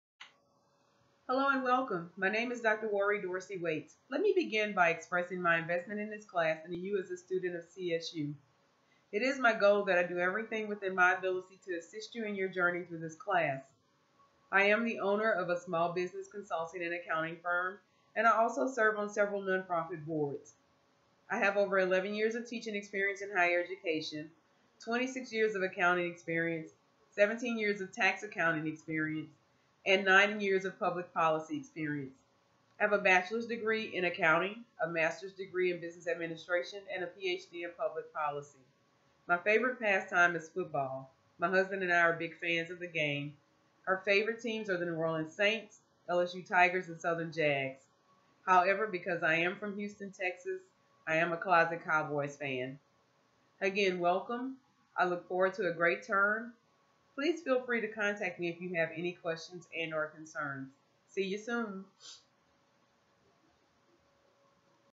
Columbia Southern University Welcome
Hello and Welcome! My Name is Dr. Waree Dorsey-Waits.  Let me begin by expressing my investment in this class and in you as a student of CSU.  It is my goal that I do everything within my ability to assist you in your journey through this class.

I am the owner of a Small Business Consulting and Accounting Firm and I also serve on several Non-Profit Boards.  I have over 11 years of teaching experience in Higher Education, 26 years of accounting experience and 17 years of tax accounting experience.  I have a Bachelor’s degree in Accounting, A masters degree in Business Administration and a PhD in Public Policy.

My favorite pastime is football.  My husband and I are big fans of the game.  Our favorite teams are the New Orleans Saints, LSU Tigers and Southern Jags.  However, because I am from Houston Texas, I am a closet Dallas Cowboys fan.

Again, welcome, I look forward to a great term.